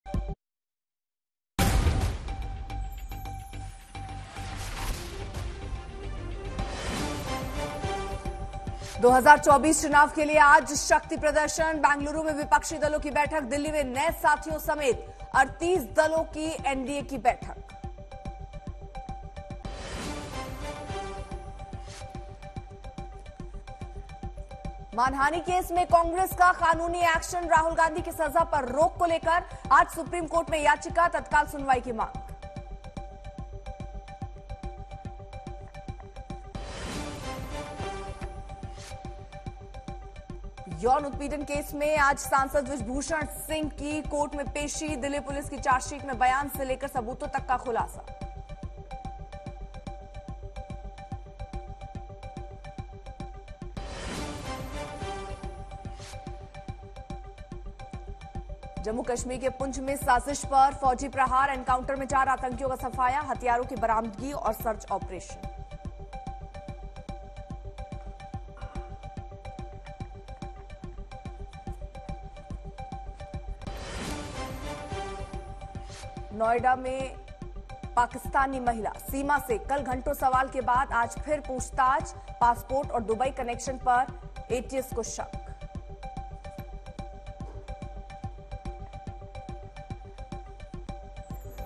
Top Headlines Of The Day: NDA Meeting |PM Modi | Amit Shah | 2024 Election | Opposition Meeting News
2024 चुनाव के लिए आज शक्ति प्रदर्शन- बैगलुरु में विपक्षी दलों की बैठक-दिल्ली में नए साथियों समेत 38 दलों की एनडीए की बैठक
मानहानि केस में कांग्रेस का कानूनी एक्शन-राहुल गांधी की सजा पर रोक को लेकर आज सुप्रीम कोर्ट में  याचिका-तत्काल सुनवाई की  मांग
यौनउत्पीडन केस में आज सांसद बृृजभूषण सिंह की कोर्ट में पेशी-दिल्ली पुलिस की चार्जशीट में  बयान से लेकर सबूतों तक का खुलासा
जम्मू कश्मीर के पूंछ में साजिश पर फौजी प्रहार- एनकाउंटर मे चार आतंकियों का सफाया- हथियारों की बरामदी और सर्च ऑपरेशन
नोएडा में पाकिस्तानी महिला सीमा से कल घंटों सवाल के बाद आज फिर पूछताछ- पासपो्ट और दुबई कनेक्शन पर एटीएस का शक !